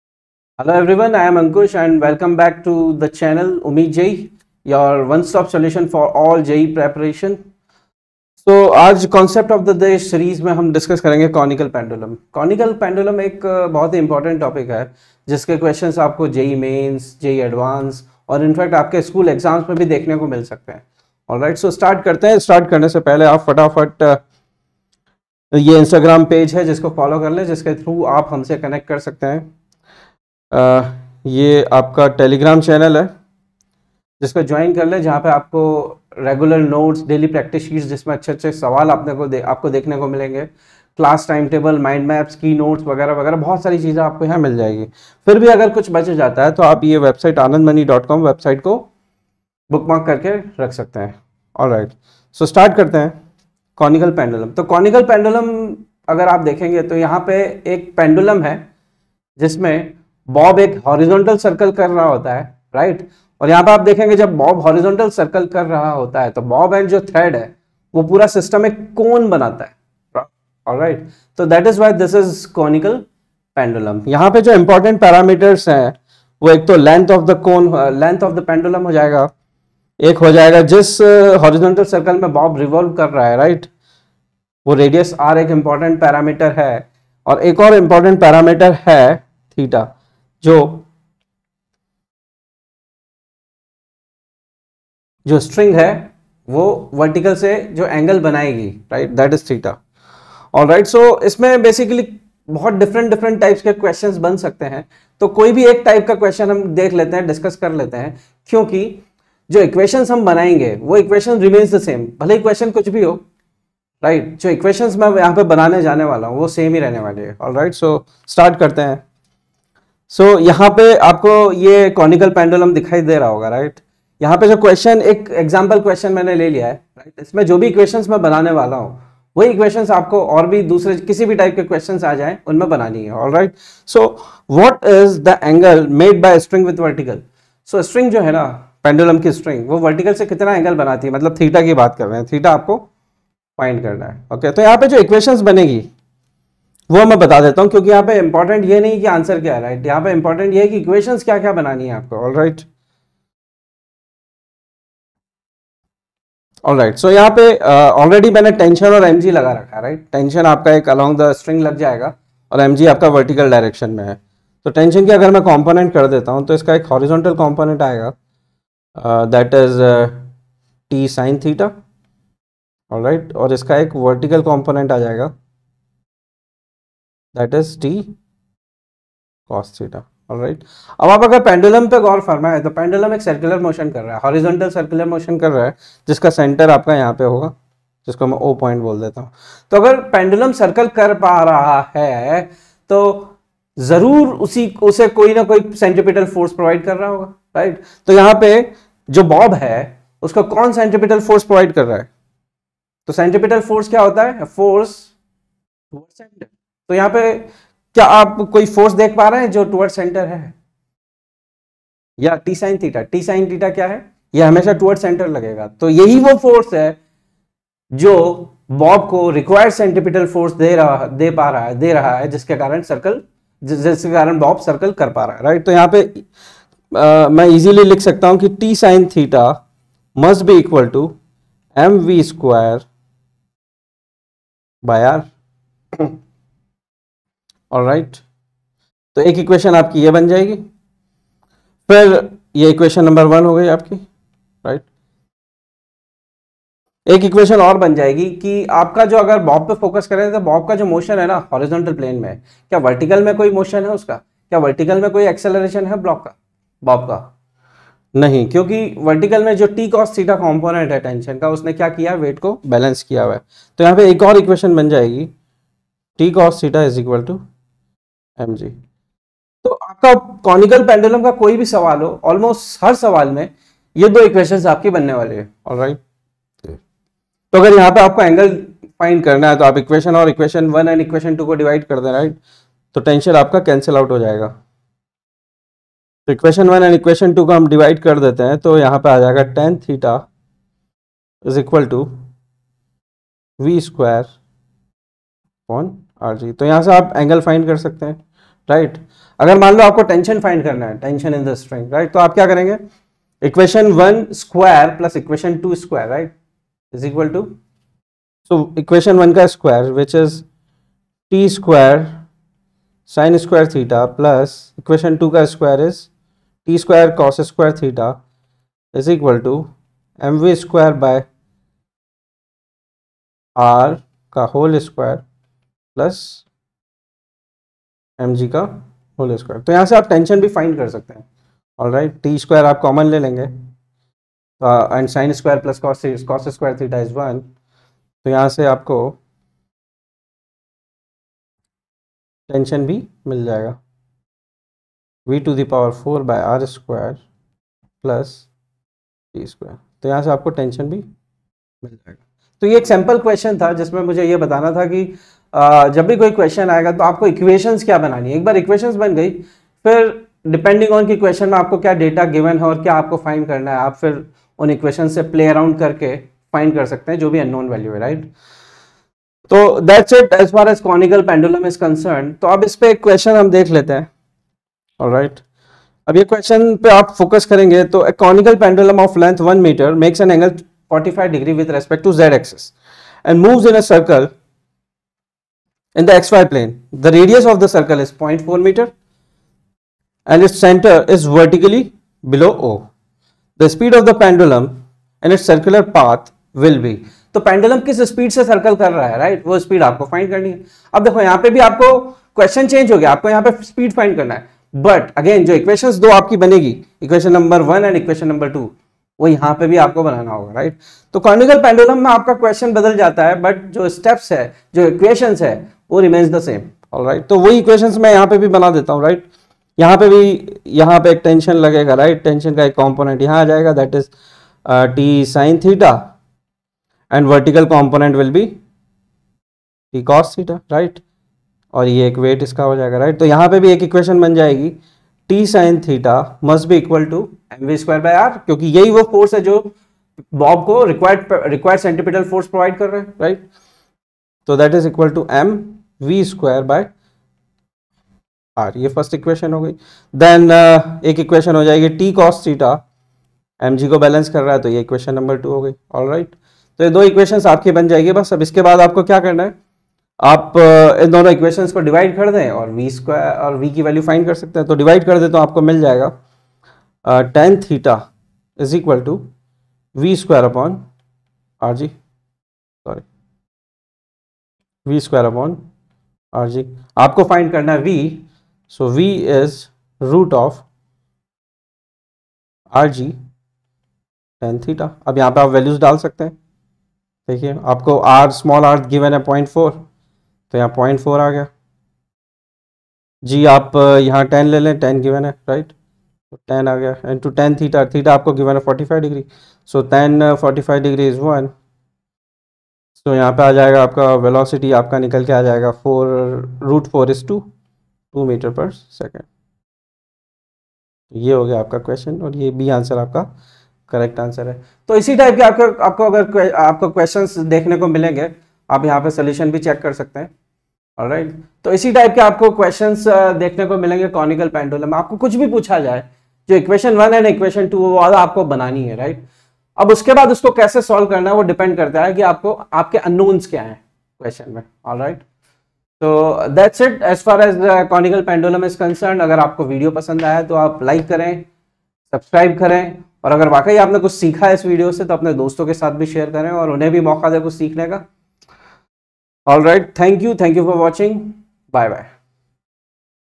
Conical Pendulum | Physics | Umeed JEE | Dr. Anand Mani and Team
In this video Ankush Sir will be discussing Conicalpendulum. Which will help students to understand and to get a clear idea of the topic. This video will benefit all JEE aspirants.